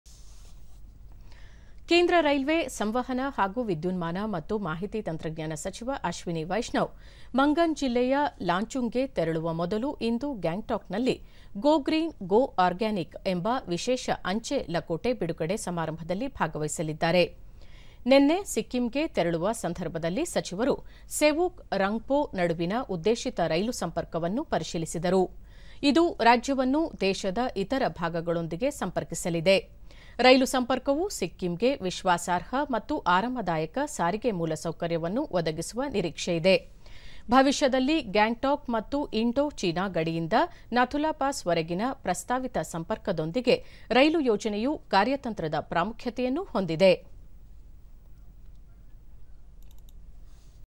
"ಗೋ ಗ್ರೀನ್, ಗೋ ಆರ್ಗಾನಿಕ್" ಅಂಚೆ ಲಕೋಟೆ ಬಿಡುಗಡೆ ಸಮಾರಂಭ ; ಕೇಂದ್ರ ರೈಲ್ವೆ ಸಚಿವ ಅಶ್ವಿನಿ ವೈಷ್ಣವ್ ಭಾಗಿ
ಕೇಂದ್ರ ರೈಲ್ವೆ, ಸಂವಹನ ಹಾಗೂ ವಿದ್ಯುನ್ಮಾನ ಮತ್ತು ಮಾಹಿತಿ ತಂತ್ರಜ್ಞಾನ ಸಚಿವ ಅಶ್ವಿನಿ ವೈಷ್ಣವ್, ಮಂಗನ್ ಜಿಲ್ಲೆಯ ಲಾಚುಂಗ್ ಗೆ  ತೆರಳುವ ಮೊದಲು ಇಂದು ಗ್ಯಾಂಗ್ಟಾಕ್ ನಲ್ಲಿ "ಗೋ ಗ್ರೀನ್, ಗೋ ಆರ್ಗಾನಿಕ್" ಎಂಬ ವಿಶೇಷ ಅಂಚೆ ಲಕೋಟೆ ಬಿಡುಗಡೆ ಸಮಾರಂಭದಲ್ಲಿ ಭಾಗವಹಿಸಲಿದ್ದಾರೆ. ನಿನ್ನೆ ಸಿಕ್ಕಿಂಗೆ ತೆರಳುವ ಸಂದರ್ಭದಲ್ಲಿ ಸಚಿವರು ಸೆವೊಕ್ - ರಂಗ್ಪೋ ನಡುವಿನ ಉದ್ದೇಶಿತ ರೈಲು ಸಂಪರ್ಕವನ್ನು ಪರಿಶೀಲಿಸಿದರು. ಇದು ರಾಜ್ಯವನ್ನು ದೇಶದ ಇತರ ಭಾಗಗಳೊಂದಿಗೆ ಸಂಪರ್ಕಿಸಲಿದೆ. ರೈಲು ಸಂಪರ್ಕವು ಸಿಕ್ಕಿಂಗೆ ವಿಶ್ವಾಸಾರ್ಹ ಮತ್ತು ಆರಾಮದಾಯಕ ಸಾರಿಗೆ ಮೂಲಸೌಕರ್ಯವನ್ನು ಒದಗಿಸುವ ನಿರೀಕ್ಷೆಯಿದೆ. ಭವಿಷ್ಯದಲ್ಲಿ ಗ್ಯಾಂಗ್ಟಾಕ್ ಮತ್ತು ಇಂಡೋ - ಚೀನಾ ಗಡಿಯಿಂದ ನಾಥು ಲಾ ಪಾಸ್ ವರೆಗೆ ಪ್ರಸ್ತಾವಿತ ಸಂಪರ್ಕದೊಂದಿಗೆ ರೈಲು ಯೋಜನೆಯು ಕಾರ್ಯತಂತ್ರದ ಪ್ರಾಮುಖ್ಯತೆಯನ್ನು ಹೊಂದಿದೆ.